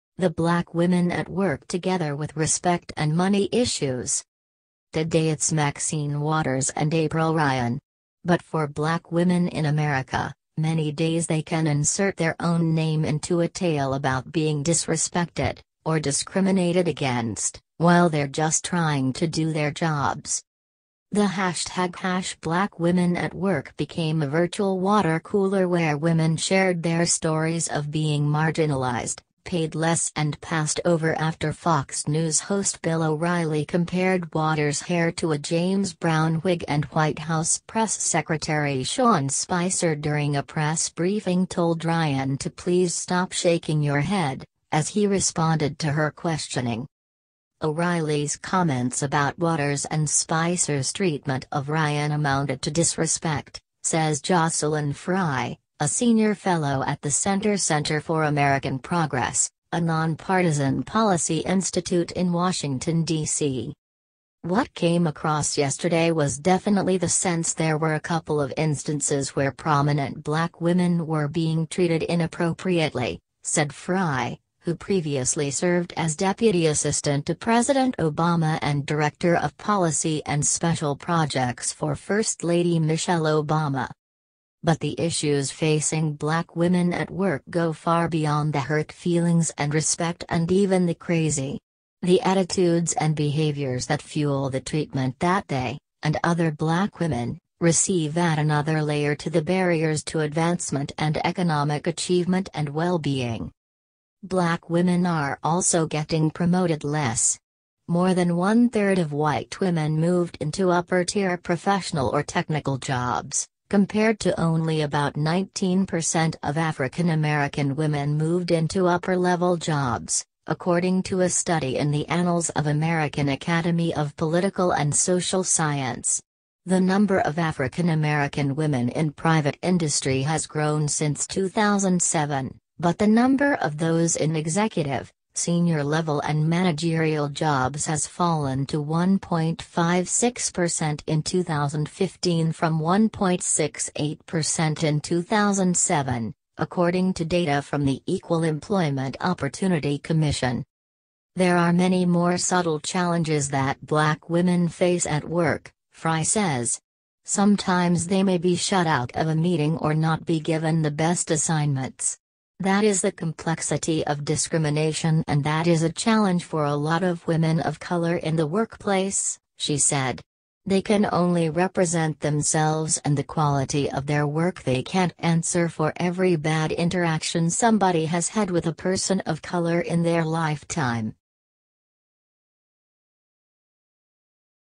The Black Women At Work Together With Respect And Money Issues
Today it's Maxine Waters and April Ryan. But for black women in America, many days they can insert their own name into a tale about being disrespected, or discriminated against, while they're just trying to do their jobs.
O'Reilly's comments about Waters and Spicer's treatment of Ryan amounted to disrespect, says Jocelyn Frye, a senior fellow at the center Center for American Progress, a nonpartisan policy institute in Washington, D.C.